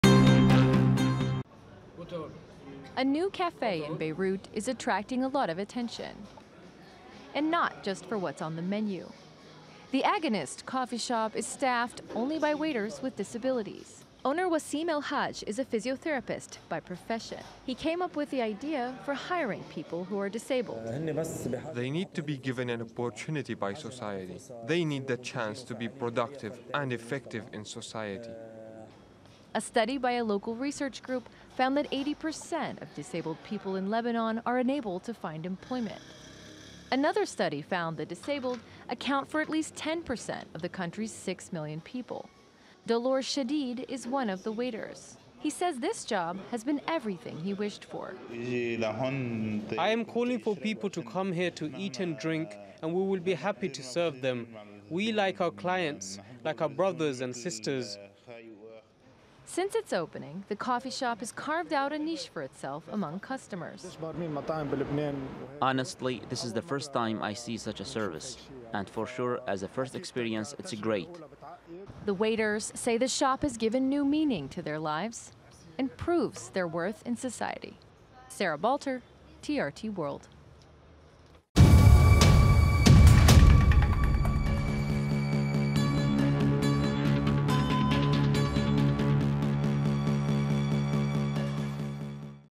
Lebanon Coffee Shop: Cafe only hires people with disabilities
In Lebanon, people with disabilities are subjected to discrimination and marginalisation, making it hard for them to find a job. But as Sarah Balter explains, a coffee shop in Beirut is trying to change things for the better.